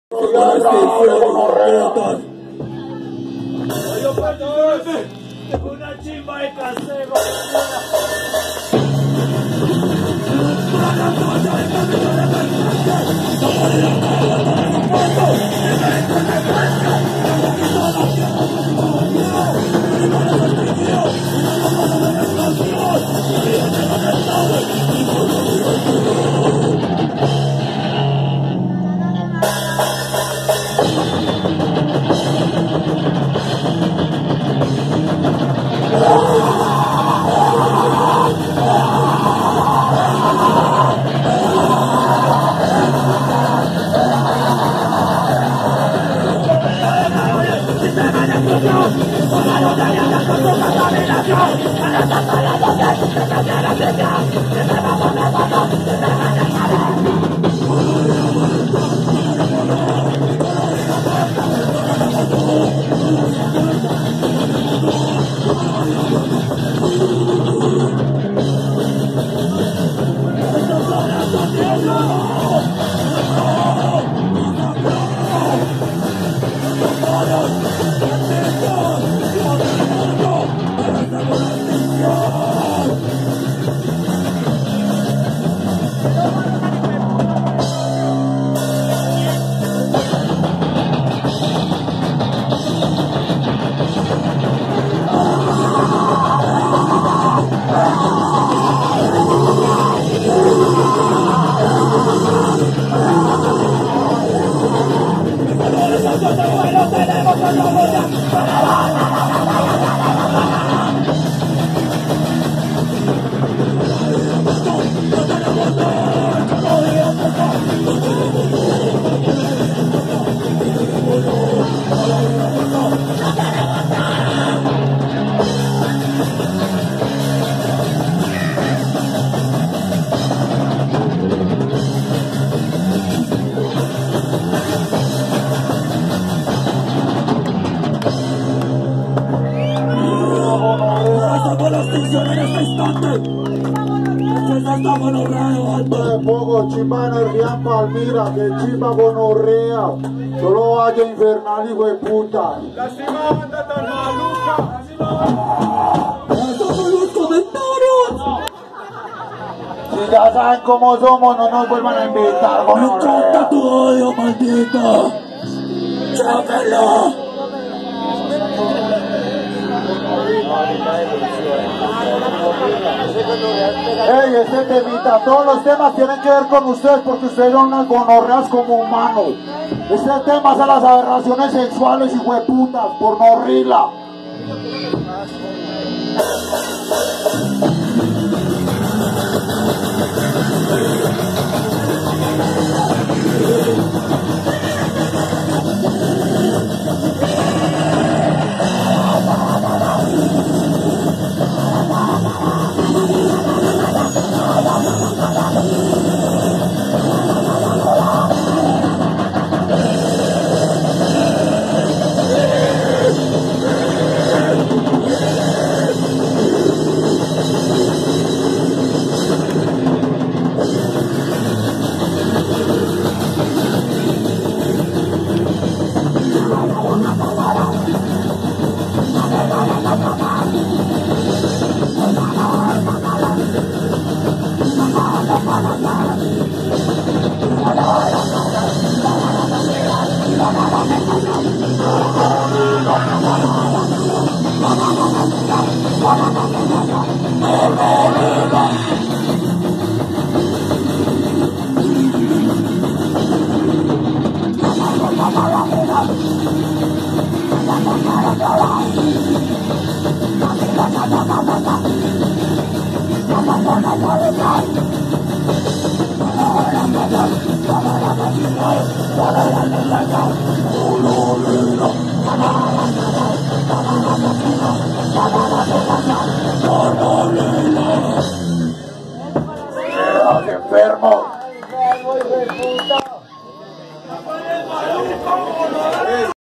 RiLA - [en vivo - PALMIRAHELL: Residuos Del Virus] - parte 2
0:00 -
3:10 -

Presentación realizada el sábado 21 de noviembre del 2020 en la ciudad de Palmira (Valle del Cauca, Colombia)
Toke: - PALMIRA HELL : RESIDUOS DEL VIRUS VOL.01
Organiza: Cinespecular - Colectivo Punk Armenia
Lugar: Casa Cultural Esneda -  Carrera 24 # 23 - 78 (Barrio El Recreo)

RILA
Banda de grindcore punk hc de la ciudad de Armenia.

Integrantes
(Guitarra y Voz) - Juan Manuel Rodriguez.
(Bajo) - Maylo Ochoa.
(Bateria) - Bryan Orozco